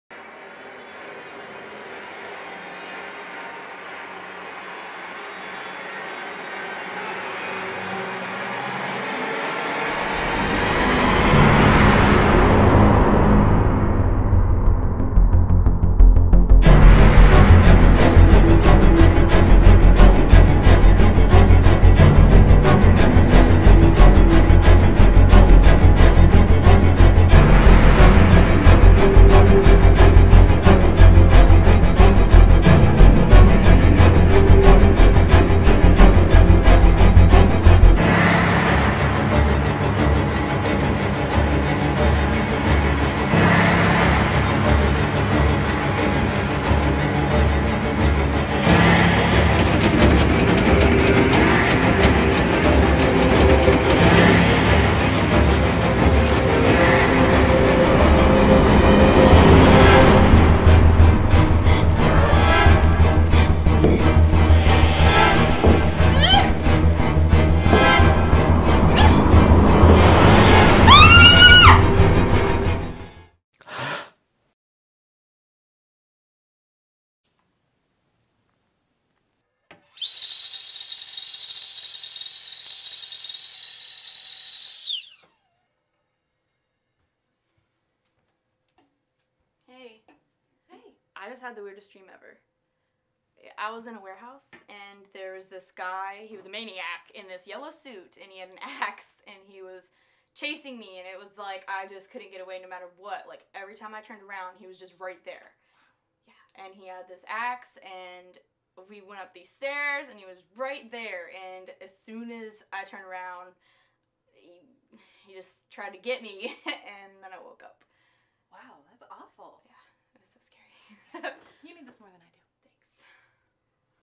Editing Exercise #1- Building a Scene
A series of exercises in which I edit a short horror film provided for educational editing purposes.